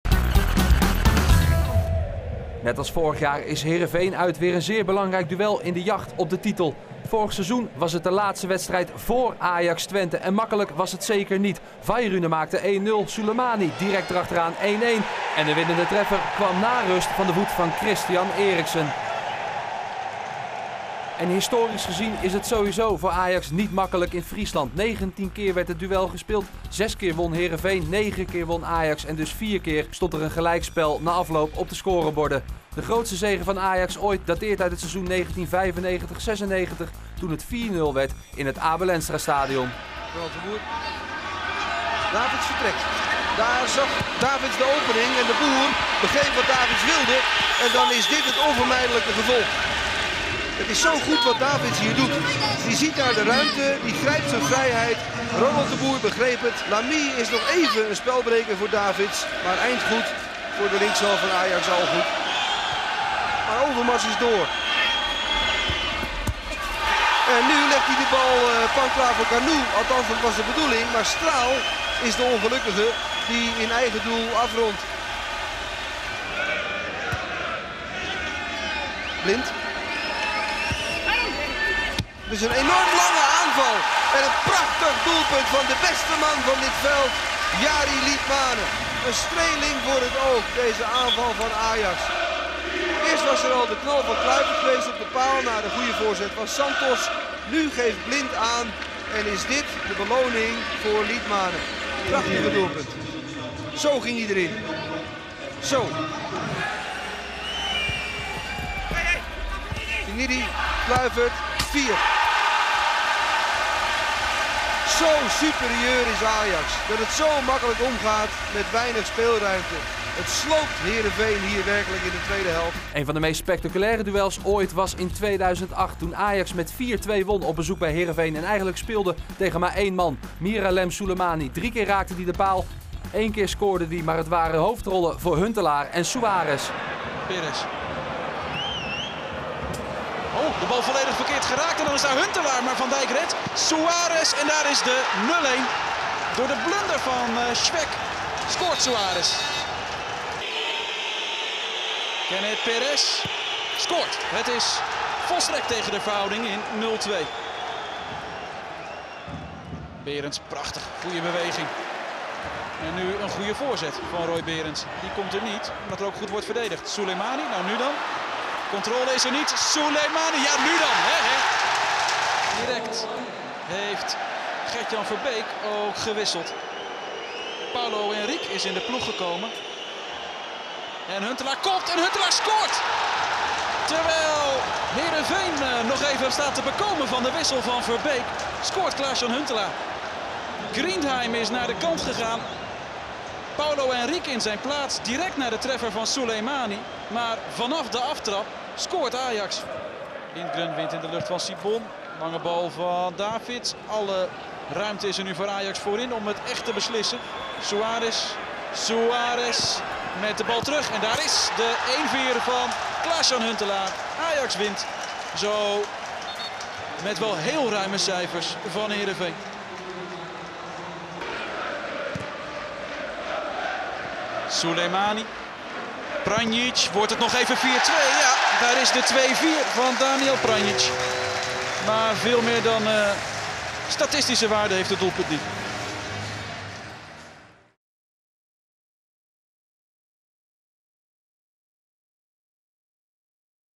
Ajax tegen Heerenveen
De uitwedstrijden van Ajax bij Heerenveen staan altijd garant voor spektakel en veel doelpunten: een overzicht uit het recente verleden.